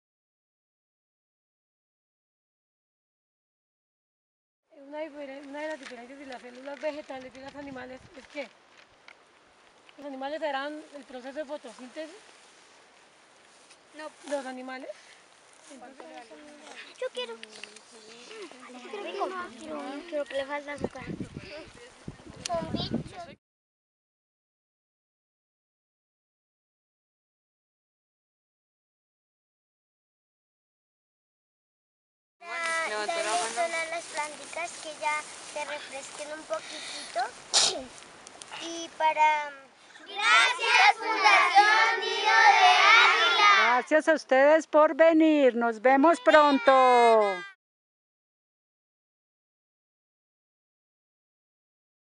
Actividades ambientales con niños de escuelas veredales
Nido de Aguilas desarrolla actividades ambientales con niños, padres de familia y profesores de la escuela veredal de Cardonal en Villa de Leyva, los chicos llegaron en busca de semillas y saberes sobre la custodia de semillas nativas. Fue un día maravilloso donde aprendimos la importancia de proteger semillas; fueron los comentarios realizados por los niños y padres de familia